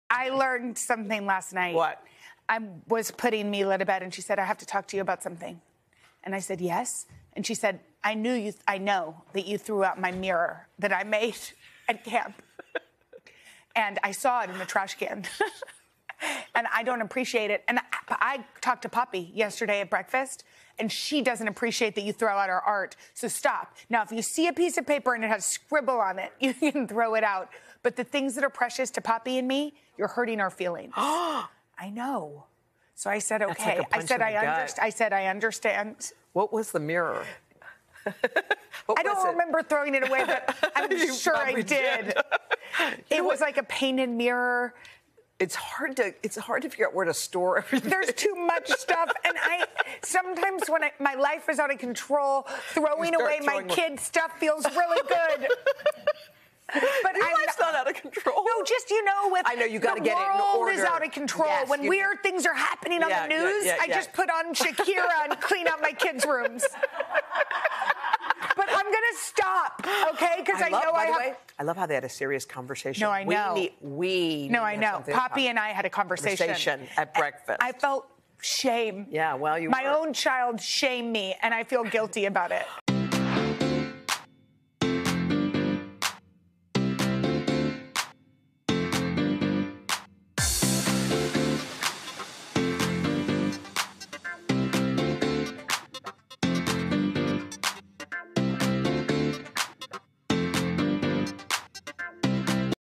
Jenna Bush Hager’s daughter confronted her for throwing away art
Jenna Bush Hager reveals that she was confronted by her daughter Mila for throwing out a mirror she made a camp and that her daughter Poppy also doesn’t appreciate when she throws out art that they make. “My own child shamed me and I feel guilty about it,” Jenna joked.